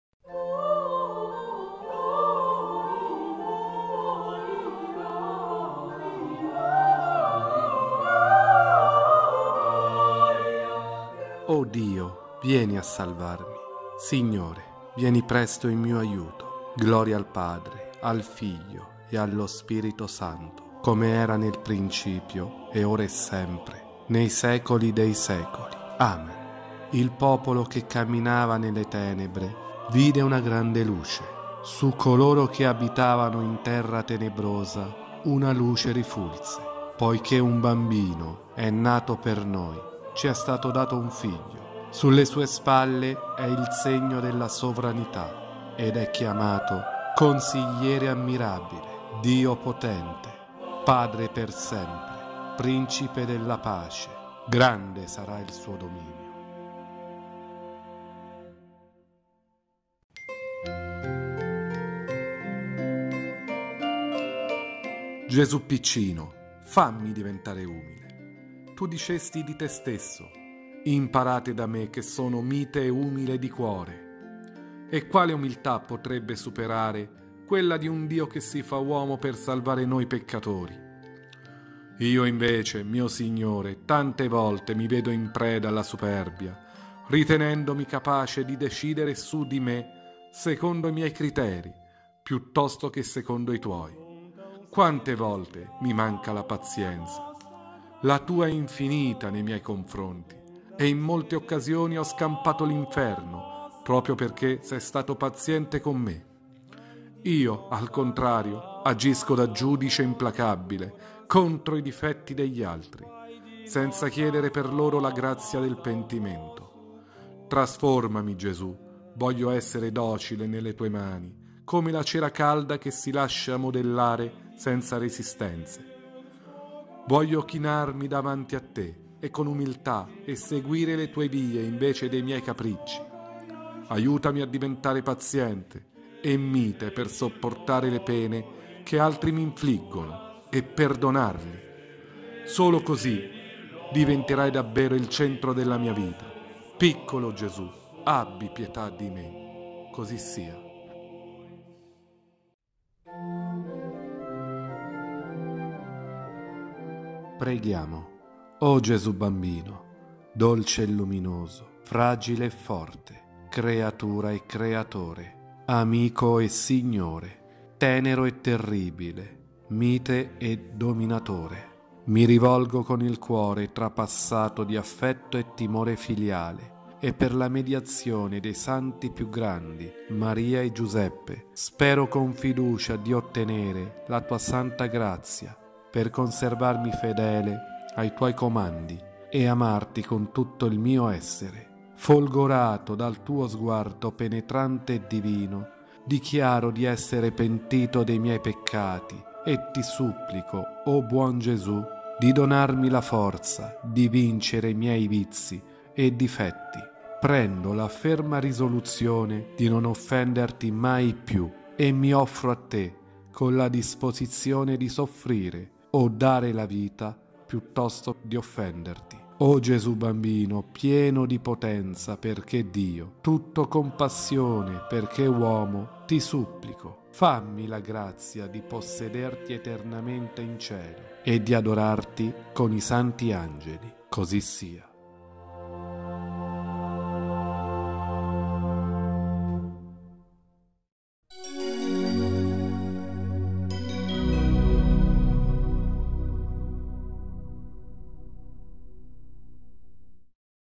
Novena di Natale - Sesto giorno - Umiltà.
Gesù piccino, fammi diventare umile. Tu dicesti di Te stesso:
Imparate da Me che sono mite e umile di cuore!